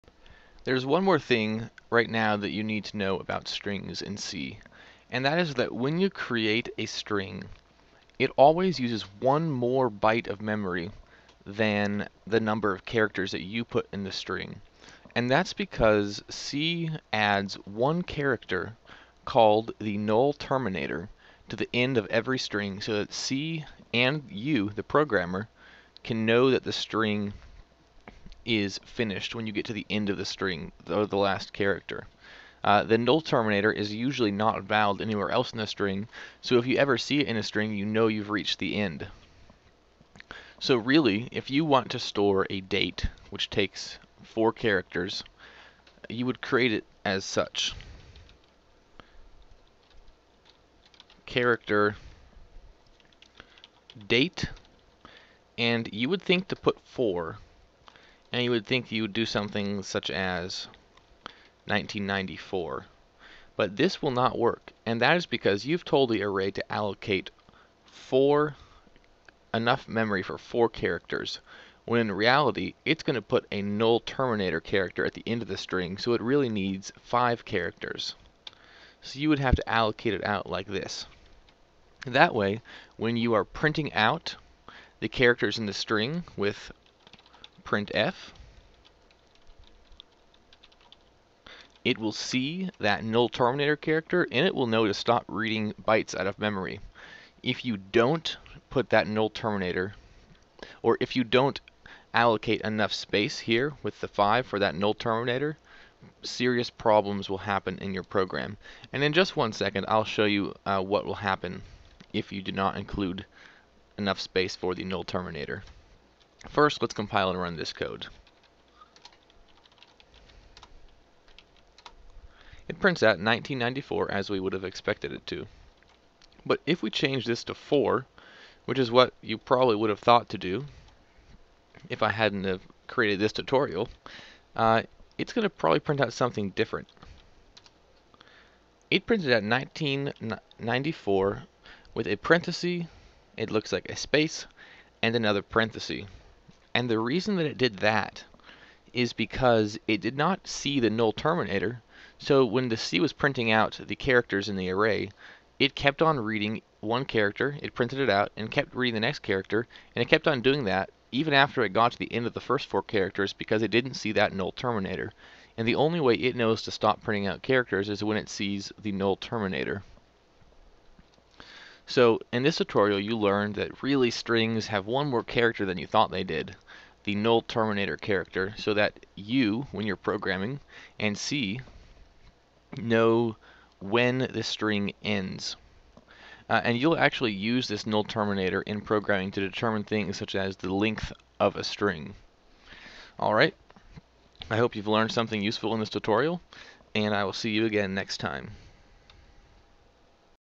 29 NUL Terminator
More about strings--covering the NUL terminator for strings.